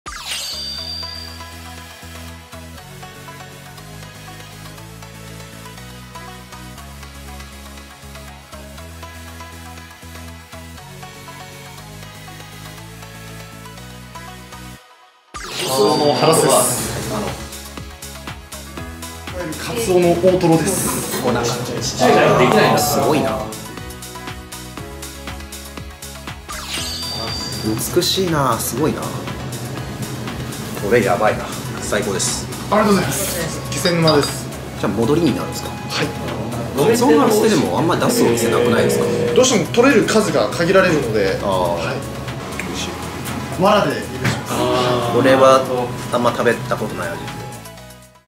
【渾身の一貫】鰹の大トロ…ハラミ【すし良月】
恵比寿の予約困難店の技法はこちらから！

【本日のお店】
東京 恵比寿「すし良月（あきら）」
おまかせ　28600円
住所：東京都渋谷区恵比寿２丁目３７−８ グランデュオ広尾 1F

[Today's shop]
Ebisu, Tokyo "Sushi Akira"
Random 28600 yen